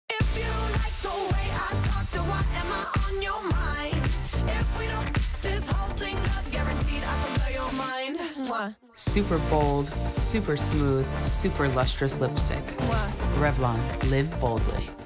Live Boldly with Super Lustrous Lipstick
Dance along with Revlon's newest ambassadors Ashley Graham, Adwoa Aboah, Imaan Hammam, Raquel Zimmerman and Rina Fukushi, as they show off Revlon's Super Lustrous Lipstick.